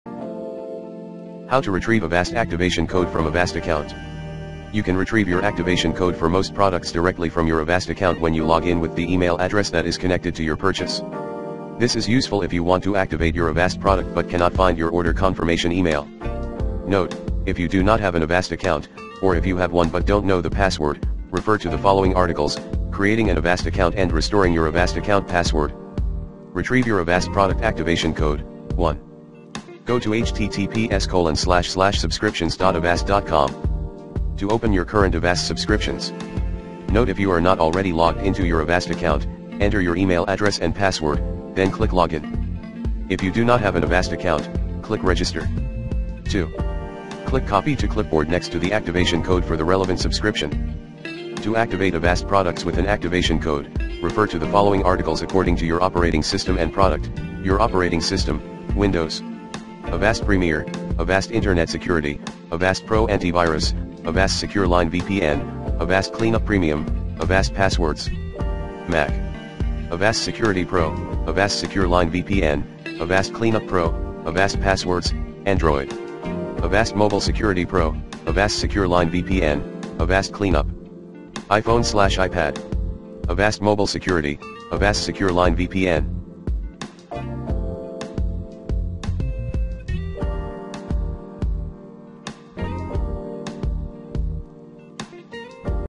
Retrieve Avast activation code from Avast Account
Here's how to Retrieve Avast activation code from Avast Account. Hit & try some top Avast app if you need.